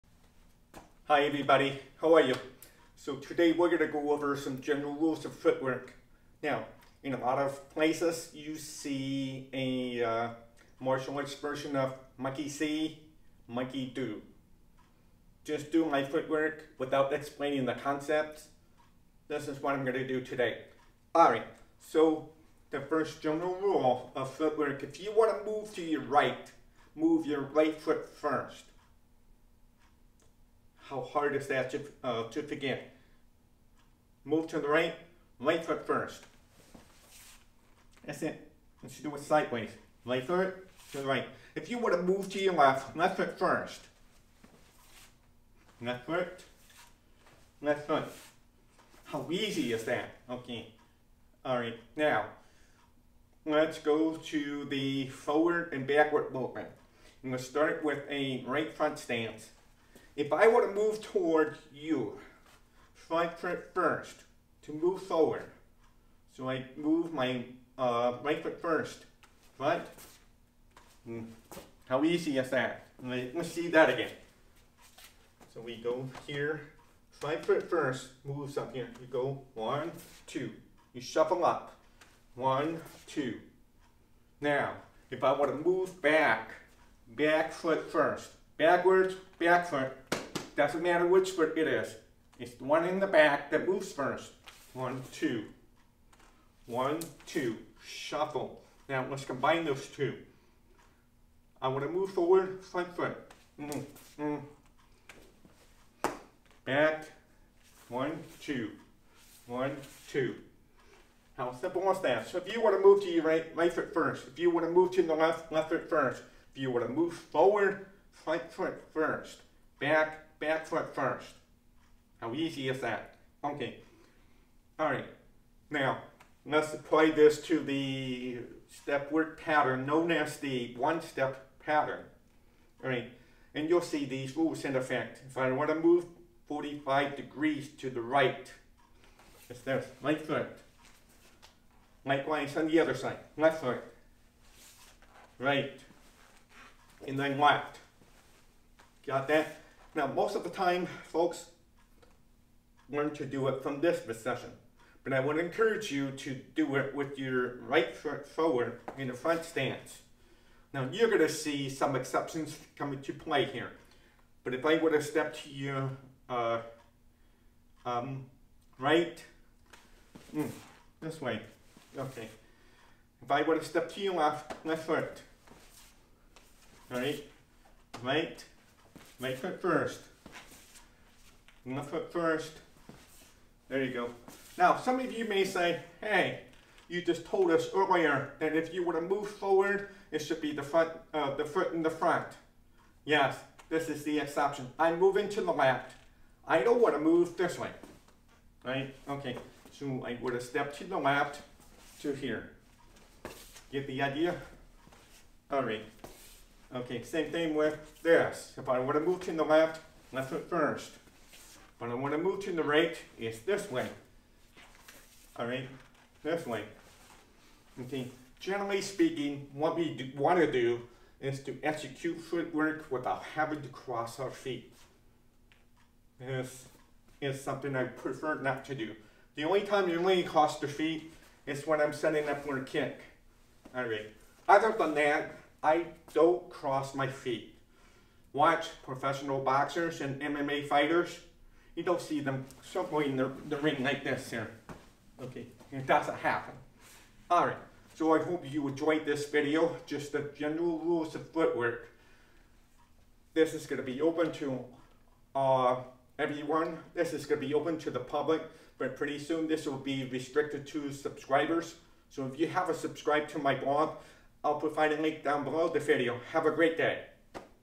Footwork Rules
I teach my students these simple footwork rules.